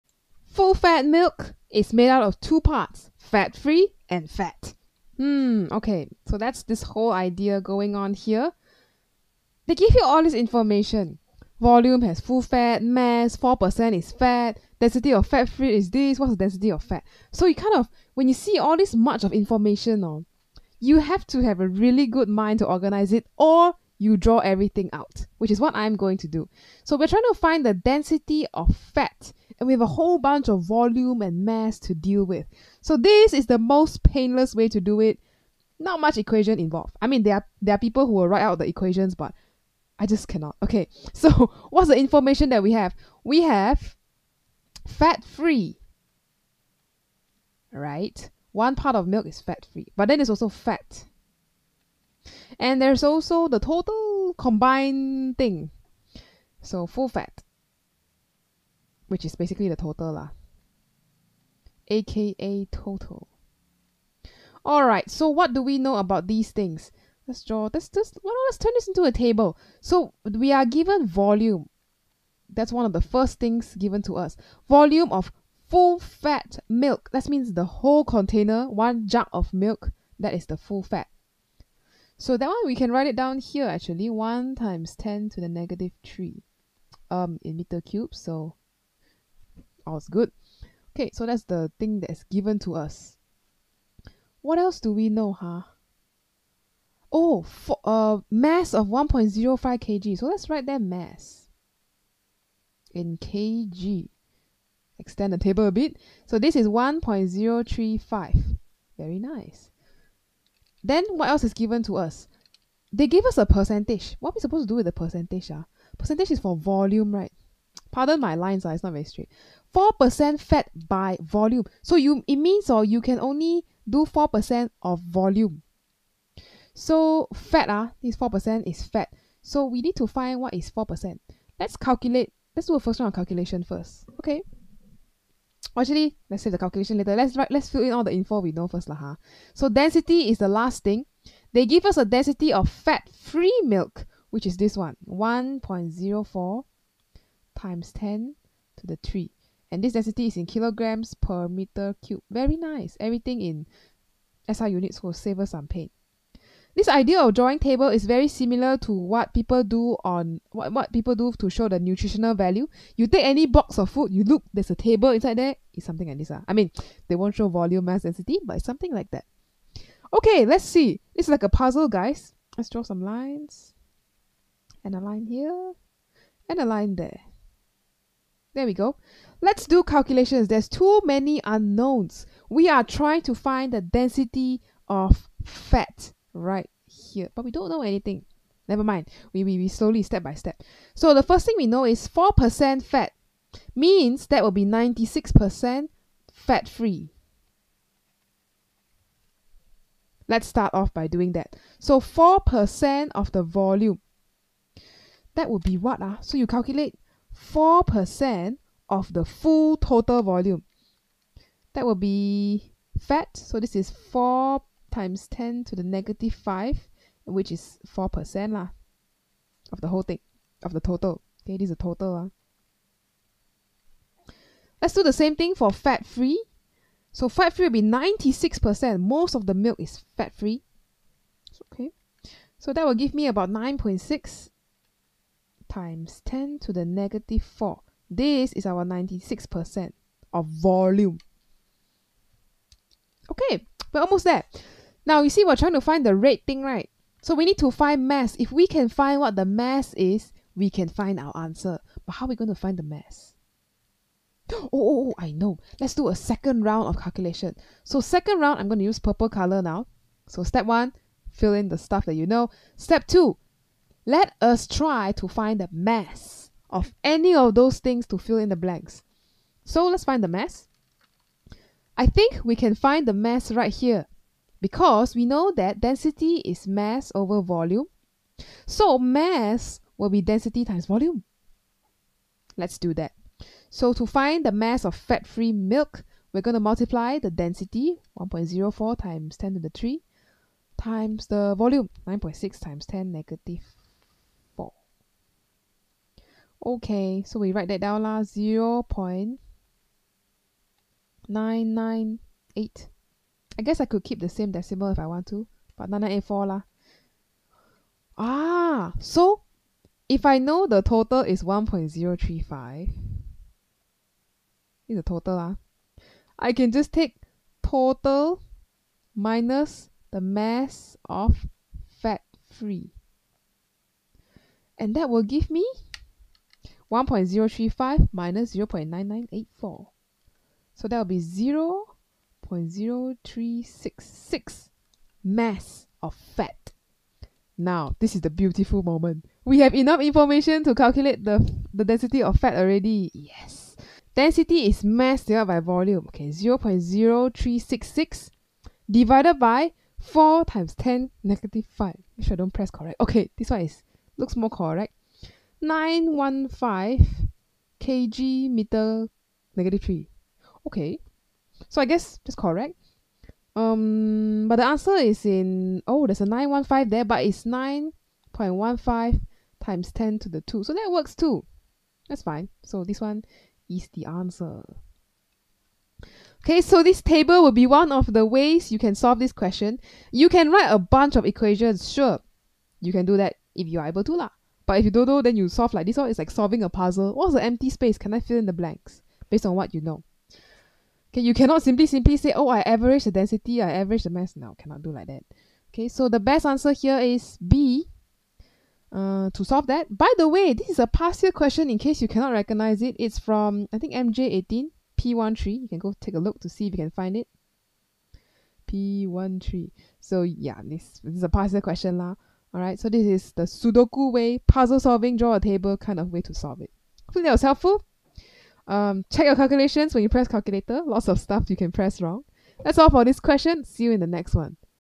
MJ18 P13 Q13 Density of Fat in Milk | May/June 2018 | CAIE A Level 9702 Physics
9702/13/M/J/18: Full-fat milk is made up of fat-free milk mixed with fat. A  volume of 1.000 × 10^–3 m^3 of full-fat milk has a mass of 1.035kg. It  contains  4.00% fat by volume.

The density of fat-free milk is 1.040 × 10^3 kg m^–3. What is the density of fat?